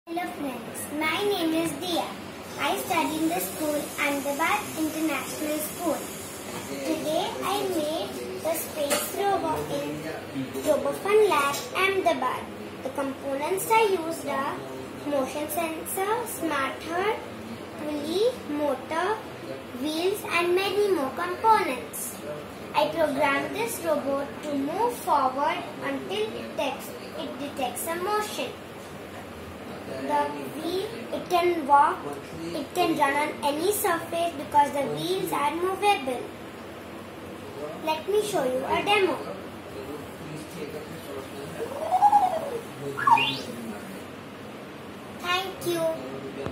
Lego Space Rover - Diya Shodhan - RFL Ahmedabad(8980172306)
Lego Space Rover using We Do 2.0 kit. It will stop using Motion sensor. It constructed & programmed By : Diya Shodhan